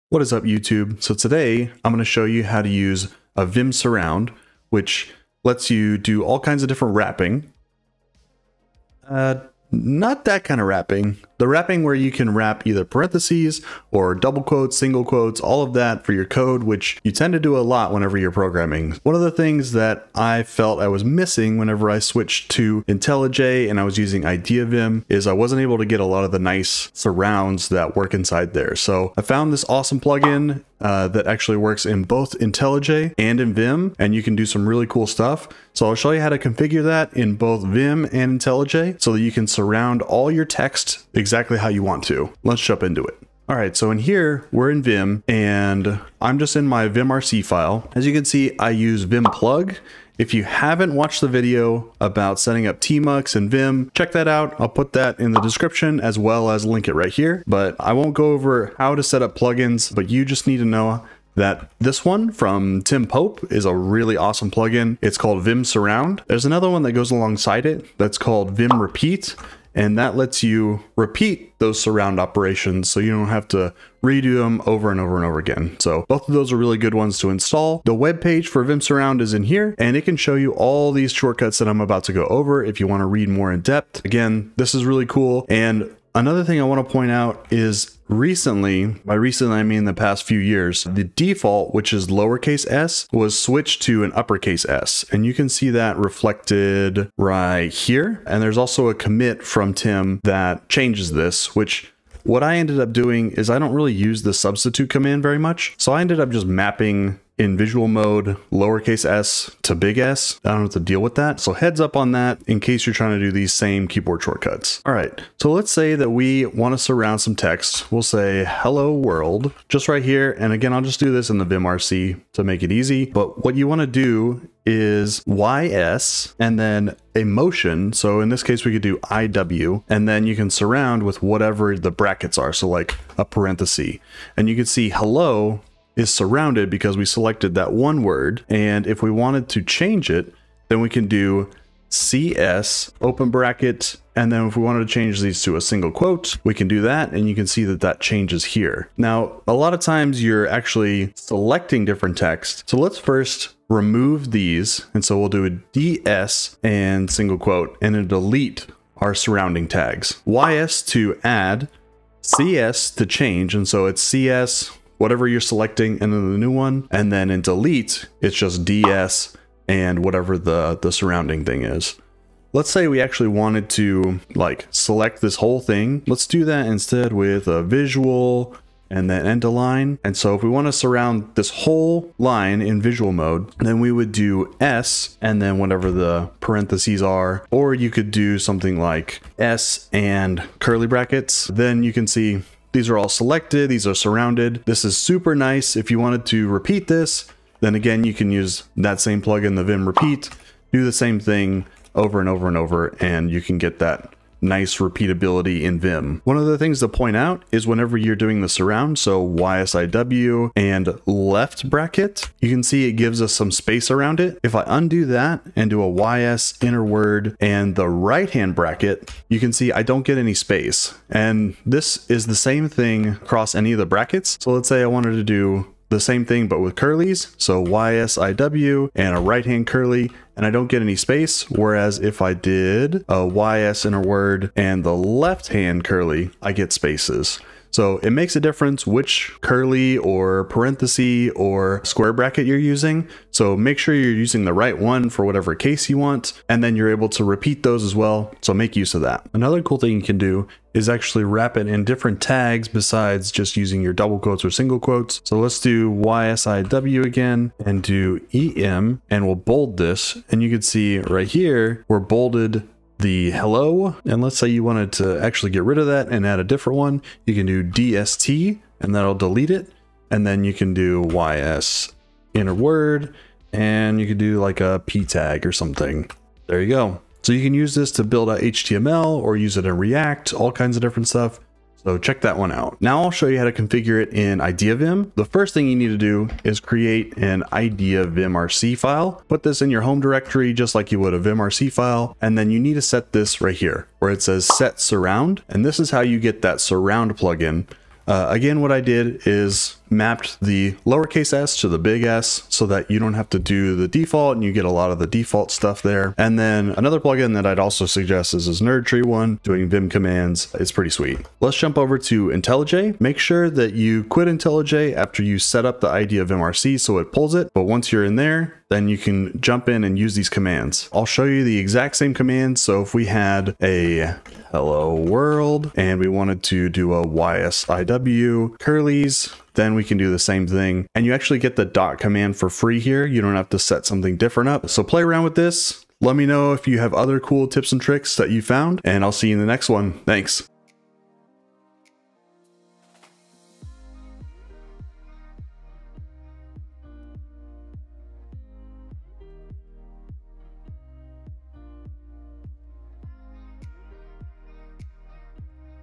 Configure vim-surround in Vim & IntelliJ
Need a better way to surround text or code blocks in Vim or using IdeaVim in IntelliJ? In this video I'll show you how to configure both using vim-surround to get some nice keyboard shortcuts to eliminate the manual text surrounding you are currently doing.

0:00 Intro
0:43 Configure vim-surround
1:54 Surrounding text in Vim
2:54 Surrounding with visual mode
4:42 Configure surround in IntelliJ
5:34 Outro